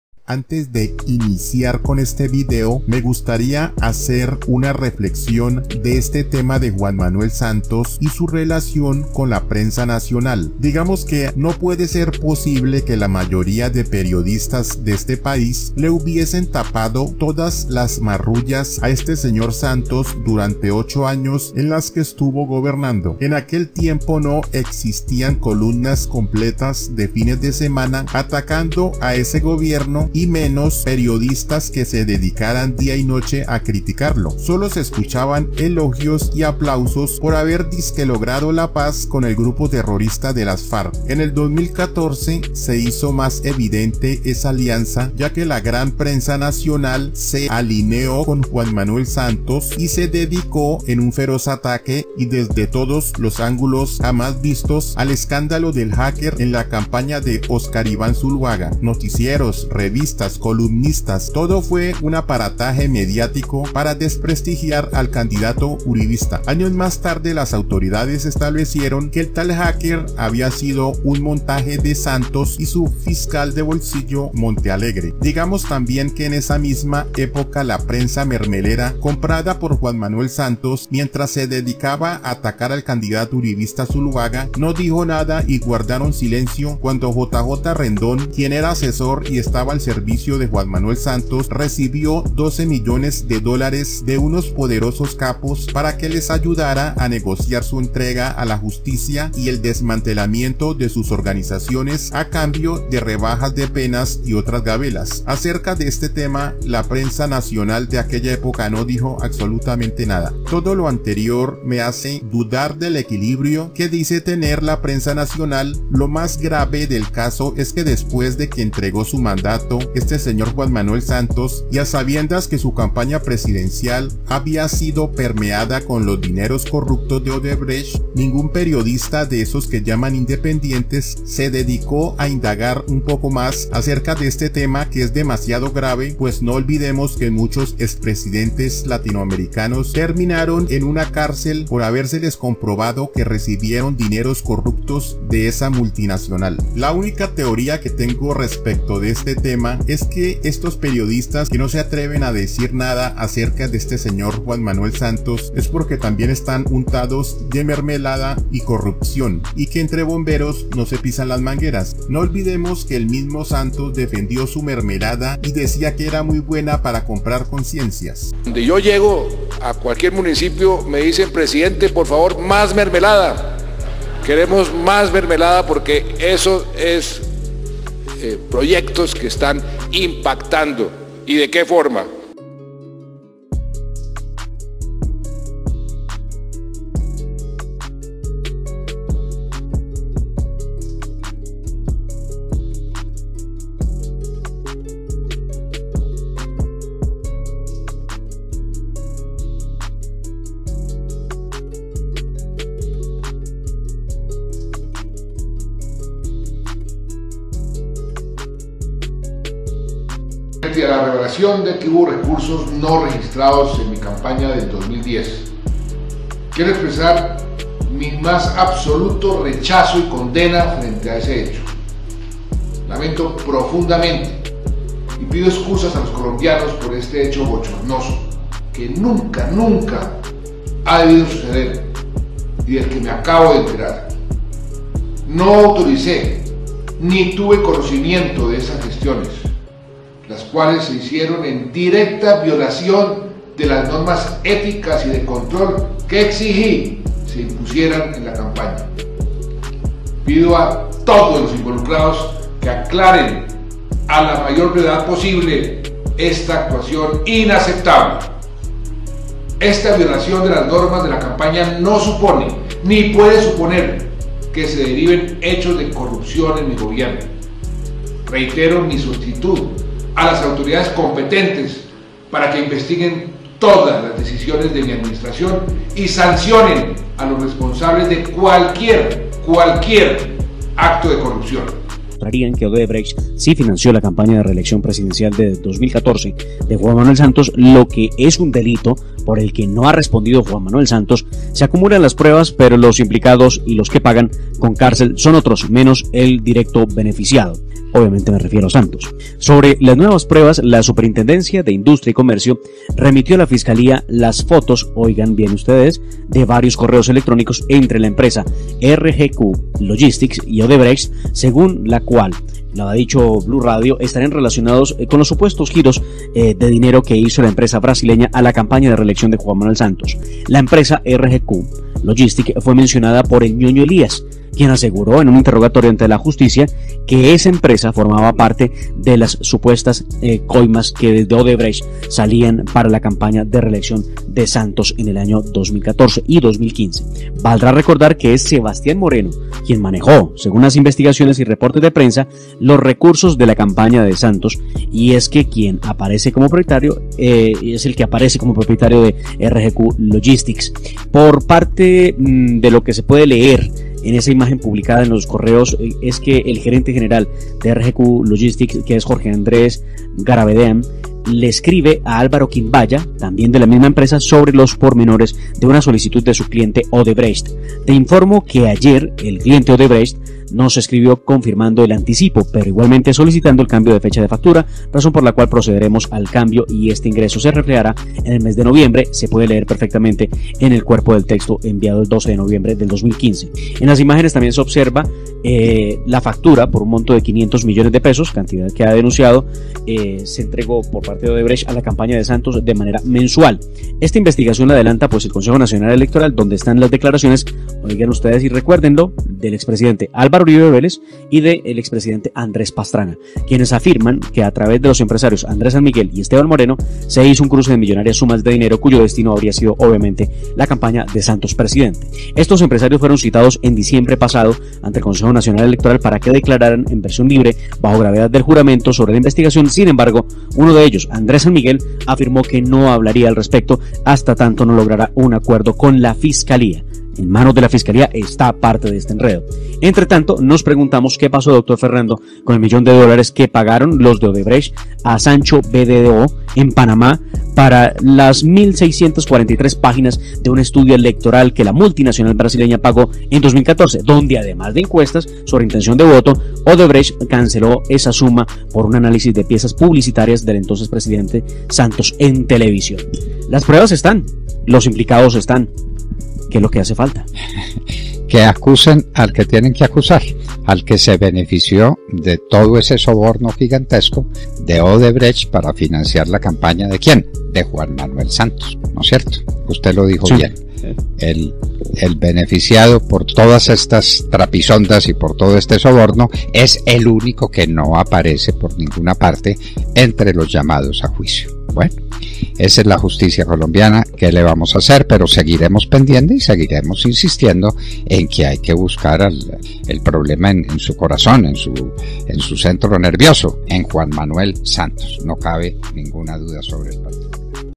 LA PRENSA VENDIDA A SANTOS Y LAS NUEVAS PRUEBAS EN SU CONTRA
En Colombia extrañamente el periodismo mermelero se abstiene de mencionar a Juan Manuel santos y en las ultimas horas se conoció una investigación divulgada por la emisora Blu radio en la que se descubrieron dos correos electrónicos que aportarían serios indicios de lo que habriá sido el despacho de giros económicos hechos por la multinacional odebrecht a la campaña de reelección del expresidente juan Manuel santos.